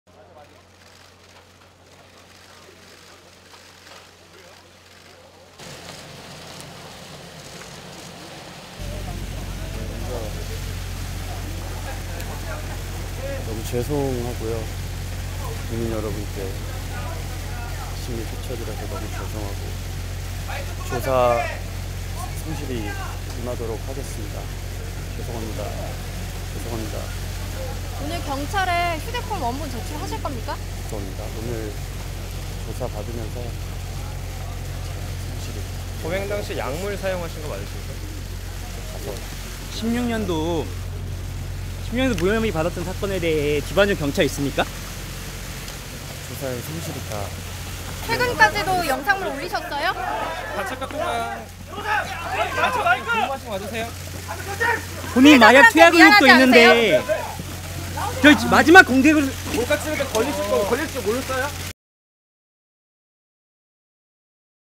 [풀영상] '몰카 논란' 정준영, 피의자 신분으로 경찰 출석…거듭 "죄송"만
(서울=뉴스1) 정윤경 기자,송영성 기자 = 성관계 영상을 상대방의 동의 없이 촬영하고 이를 단체 대화방에 유포한 혐의로 입건된 가수 정준영(30)이 14일 경찰에 출석했다.

정준영은 이날 오전 9시59분쯤 서울 종로구 서울지방경찰청 광역수사대에 피의자 신분으로 출석했다.

정준영은 "국민 여러분께 심려 끼쳐드려서 너무 죄송하고 조사 성실히 임하도록 하겠습니다"고 말했다. 이어지는 질문에는 "성실히 조사를 받겠다"라고만 했다.

이날 오후에는 성접대 혐의(성매매 알선 등 행위의 처벌에 관한 법률 위반)를 받는 가수 승리가 출석할 예정이다. 단체 대화방에 있던 유리홀딩스 대표 유모씨(34) 역시 이날 오후 경찰 조사를 받는다.